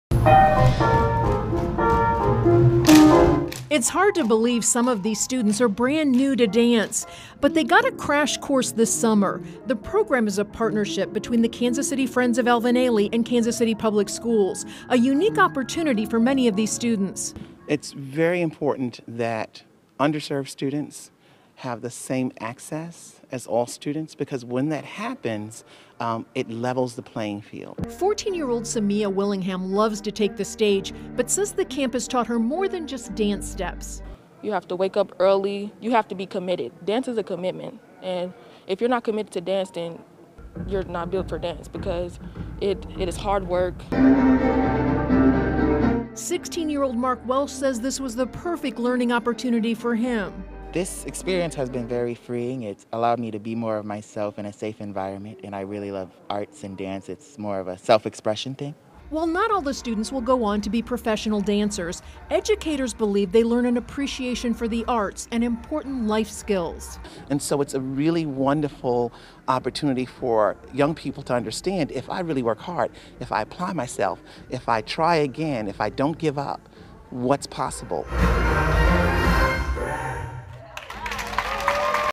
Kansas City graduates of summer school dance program show off skills
Graduates of a unique summer school program took the stage Thursday, showing off what they've learned in a five-week dance intensive. It's designed to bring the arts to underserved students and unite the community through dance.